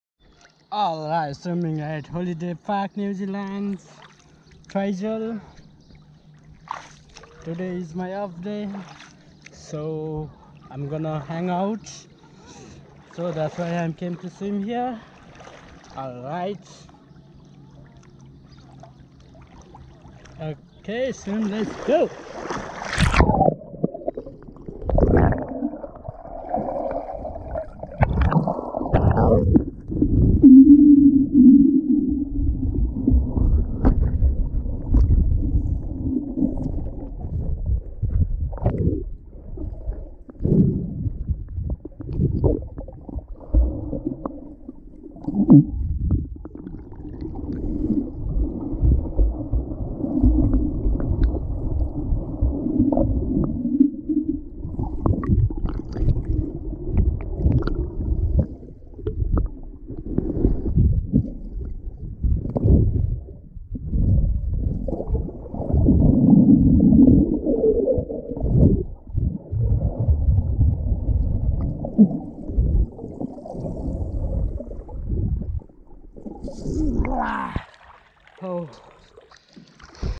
Swimming in lake Ruataniwha in New Zealand.
On a beautiful sunny day I spent the day swimming in lake Ruataniwha in Twizel in New Zealand. The lake is in a quiet, serene place and is perfect to swim. the water is not too cold and is perfect for the weather.